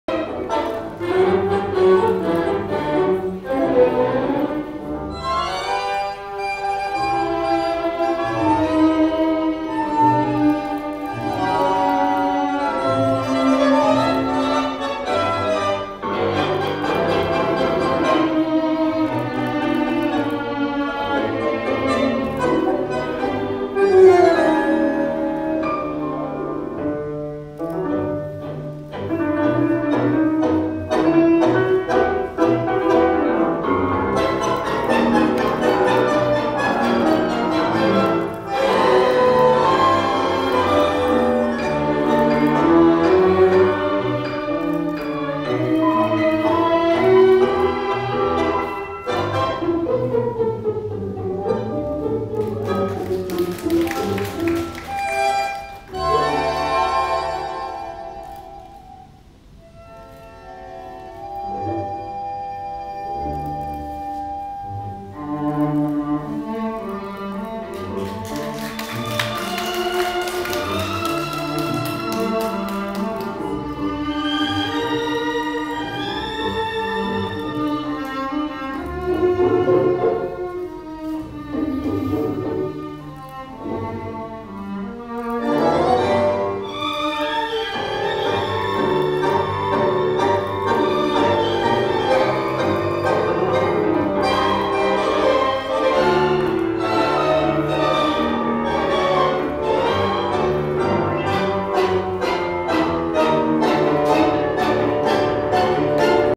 Ezequiel Paludi & Geraldine Rojas Trailer show
Noches de Luna, parts of show Ezequiel Paludi & Geraldine Rojas, Voronezh 2011 4th of June